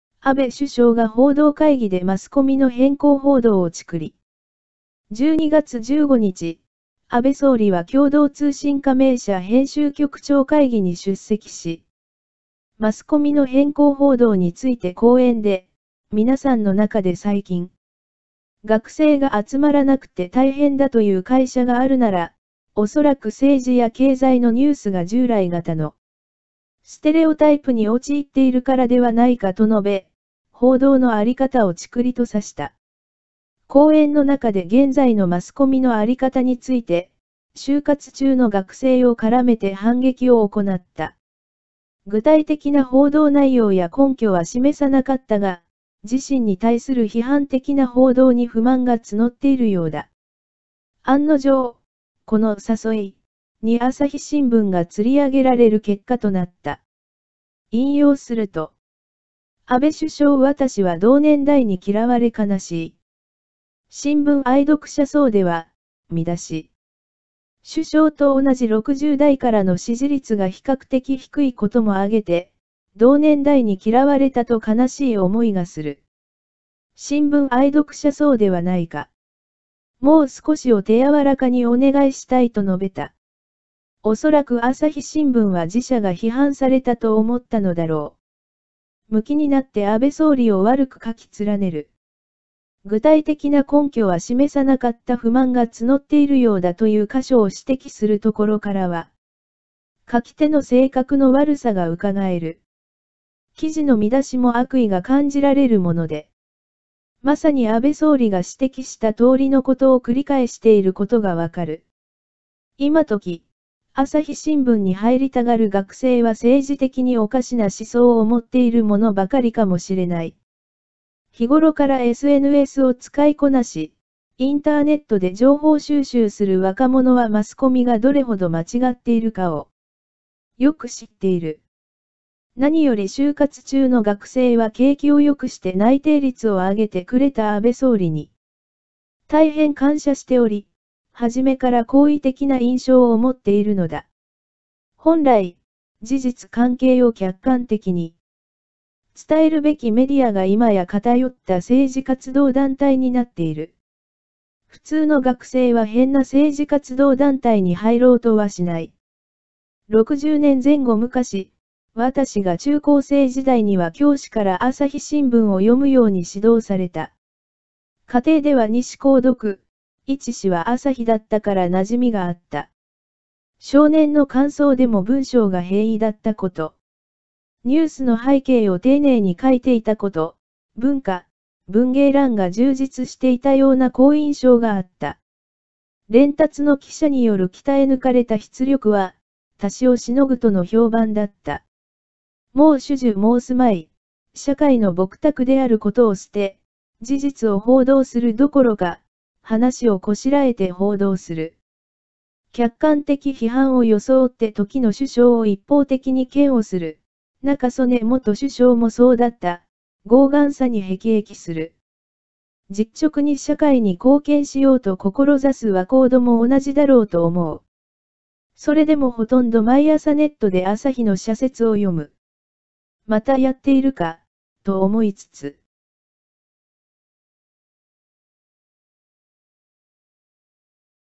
安倍首相が報道会議でマスコミ偏向報道を皮肉る
12月15日、安倍総理は共同通信加盟社編集局長会議に出席し、マスコミの偏向報道について講演で、「皆さんの中で最近、学生が集まらなくて大変だという会社があるなら、恐らく政治や経済のニュースが従来型のステレオタイプに陥っているからではないか」と述べ、報道のあり方をチクリと刺した。講演の中で現在のマスコミのあり方について、就活中の学生を絡めて反撃を行った。